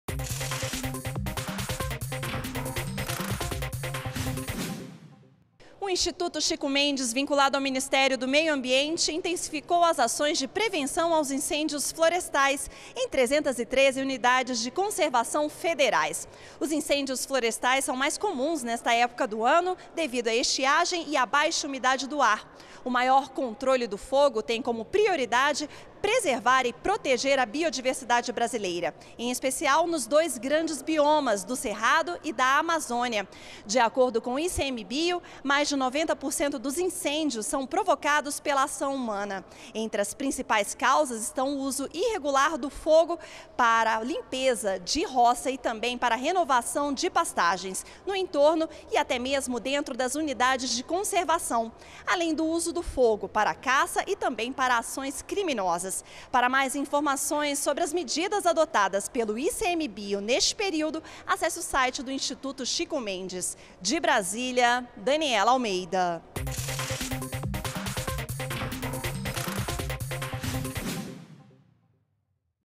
Ações para prevenir incêndios florestais são intensificadas no Cerrado e na Amazônia
ÚLTIMAS - 23.07.14: O Instituto Chico Mendes de Conservação da Biodiversidade (ICMBio) intensificou as ações de prevenção a incêndios florestais em 313 Unidades de Conservação Federais. Os incêndios florestais são mais comuns nesta época do ano devido a estiagem e a baixa umidade do ar. As ações de prevenção têm como prioridade preservar e proteger a biodiversidade brasileira, em especial, os biomas do Cerrado e da Amazônia. Segundo o Instituto, mais de 90% dos incêndios são causados por ação do homem.